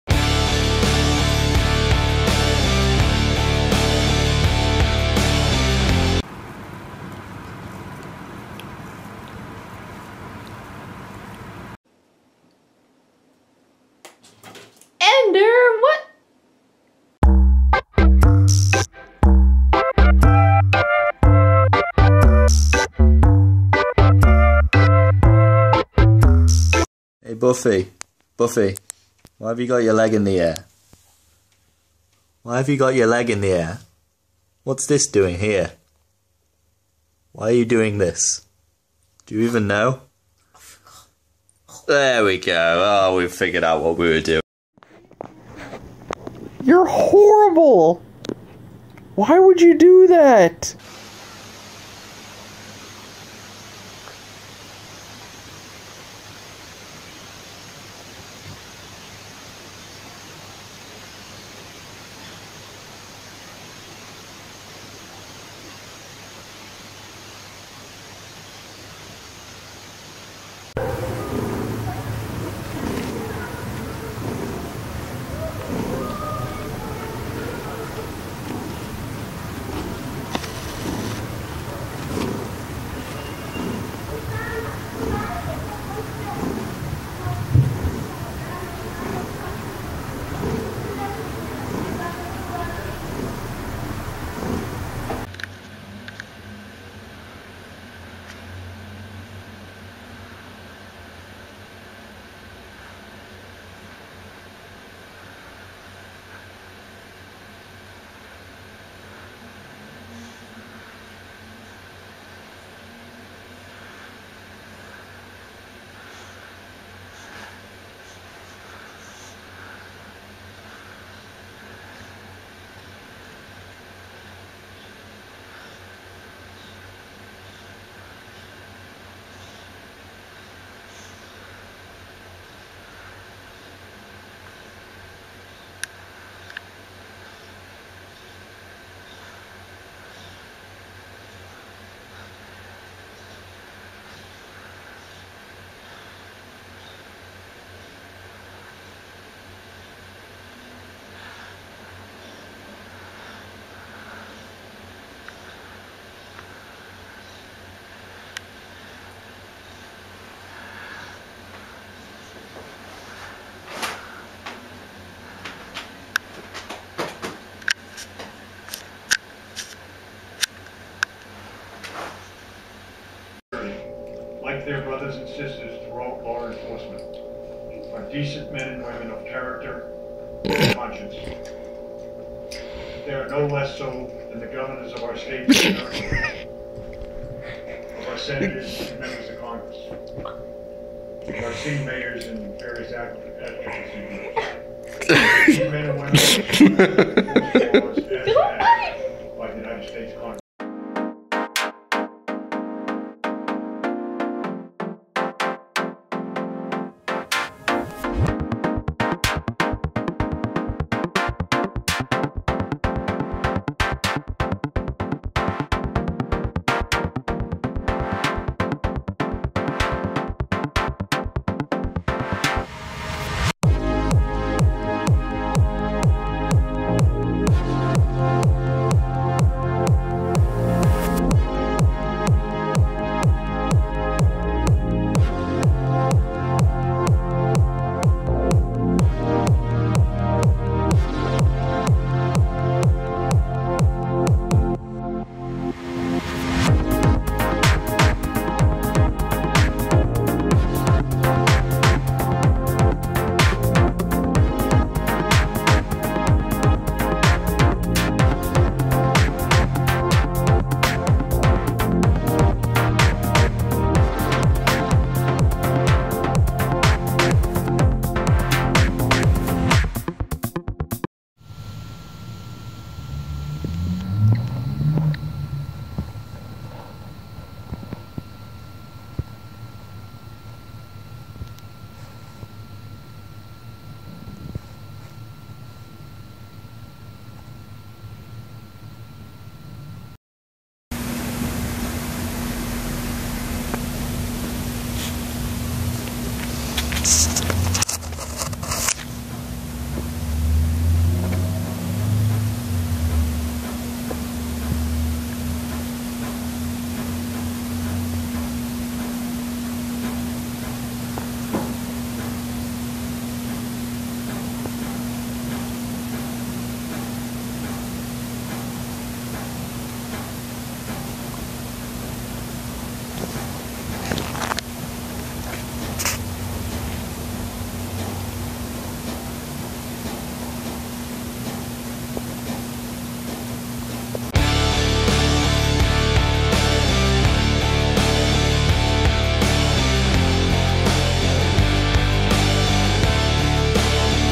Inappropriate Cats | Funny Cat Video Compilation 2017
From cats massaging themselves, cats licking stuffed animals, to awkward cats forgetting how to clean themselves, these are just a few of the inappropriate cats you'll find in this inappropriate cats video compilation.